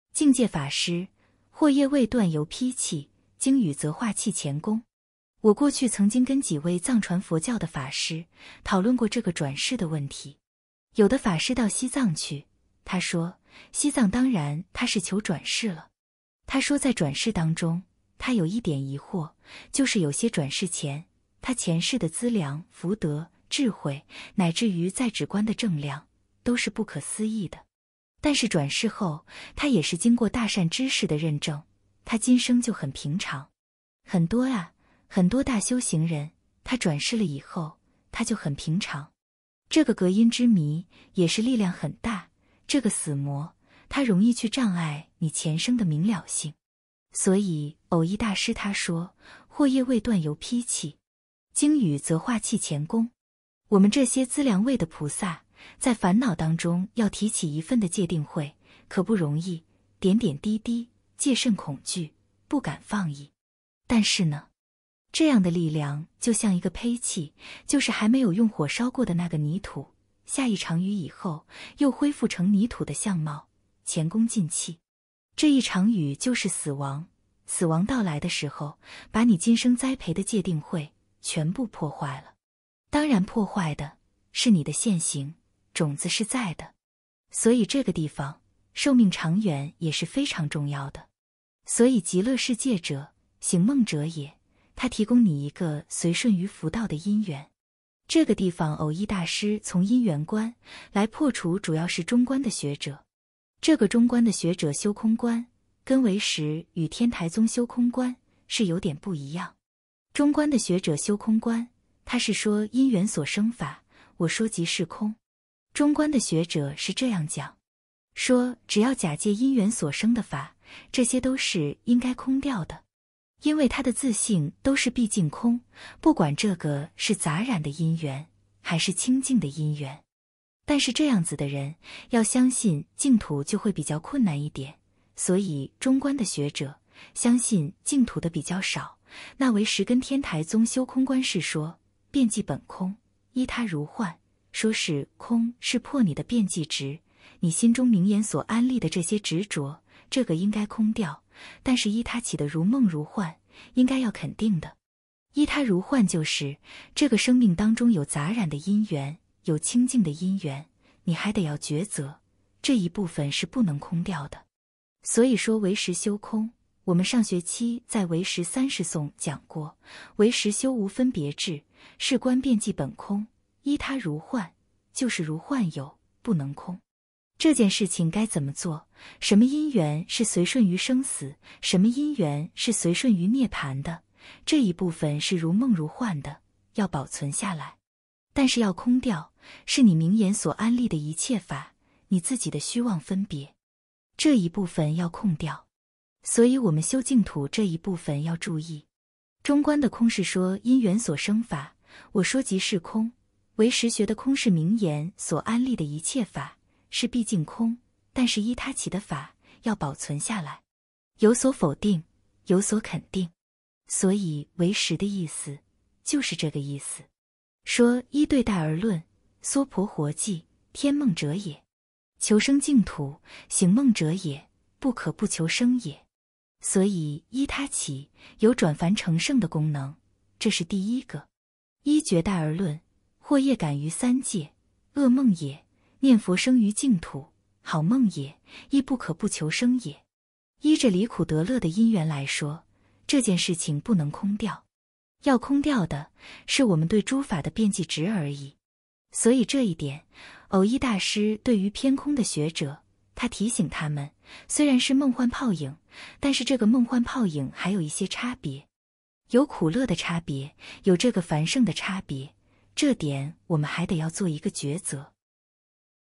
淨界法師：【惑業未斷猶坯器，經雨則化棄前功】
我過去曾經跟幾位藏傳佛教的法師，討論過這個轉世的問題。有的法師到西藏去，他說，西藏當然他是求轉世了。他說在轉世當中，他有一點疑惑，就是有些轉世前，他前世的資糧、福德、智慧，乃至於在止觀的證量，都是不可思議的。但是轉世後，他也是經過大善知識的認證，他今生就很平常。很多啊，很多大修行人，他轉世了以後，他就很平常。這個隔陰之迷，也是力量很大，這個死魔， 它容易去障礙你前生的明了性。

所以蕅益大師他說：“惑業未斷猶坯器。經雨則化棄前功。”。我們這些資糧位的菩薩，在煩惱當中要提起一份的戒定慧，可不容易！點點滴滴、戒慎恐懼、不敢放逸。但是呢，這樣的力量就像一個胚器，就是還沒有用火燒過的那個泥土，下一場雨以後，又恢復成泥土的相貌，前功盡棄。

這一場雨就是死亡，死亡到來的時候，把你今生栽培的戒定慧，全部破壞了。當然破壞的， 是你的現行，種子是在的。所以這個地方，「壽命長遠」也是非常重要的。所以“極樂世界者，醒夢者也”，它提供你一個隨順於佛道的因緣。

這個地方蕅益大師從因緣觀，來破除主要是中觀的學者。這個中觀的學者修空觀，跟唯識與天台宗修空觀，是有點不一樣。中觀的學者修空觀，他是說“因緣所生法，我說即是空”。中觀的學者是這樣講。說只要假借因緣所生的法，這些都是應該空掉的。因為它的自性都是畢竟空，不管這個是雜染的因緣、還是清淨的因緣。

但是這樣子的人，要相信淨土就會比較困難一點，所以中觀的學者，相信淨土的比較少，那唯識跟天台宗修空觀是說：“遍計本空，依它如幻”，說是“空”是破你的遍計執，你心中名言所安立的這些執著、這個應該空掉，但是依它起的“如夢如幻”，應該要肯定的。“依它如幻”就是，這個生命當中有雜染的因緣、有清淨的因緣，你還得要決擇，這一部分是不能空掉的。

所以說唯識修空，我們上學期在《唯識三十頌》講過，唯識修無分別智，是觀「遍計本空，依它如幻」，就是如幻有、不能空。這件事情該怎麼做，什麼因緣是隨順於生死、什麼因緣是隨順於涅槃的，這一部分是如夢如幻的，要保存下來。但是要空掉，是你名言所安立的一切法，你自己的虛妄分別，這一部分要空掉。

所以我們修淨土這一部分要注意。中觀的空是說“因緣所生法，我說即是空”；唯識學的「空」是「名言」所安立的一切法，是畢竟空，但是「依它起」的法，要保存下來。有所否定，有所肯定。所以唯識的意思，就是這個意思。說“依對待而論，娑婆活計，添夢者也。求生淨土，醒夢者也，不可不求生也。”。

所以依他起，有轉凡成聖的功能，這是第一個。“依絕待而論，惑業感於三界，惡夢也，念佛生於淨土，好夢也，亦不可不求生也！”依著離苦得樂的因緣來說，這件事情不能空掉。要空掉的，是我們對諸法的遍計執而已。

所以這一點，蕅益大師對於偏空的學者，他提醒他們，雖然是夢幻泡影，但是這個夢幻泡影還有一些差別，有苦樂的差別，有這個凡聖的差別，這點我們還得要做一個抉擇。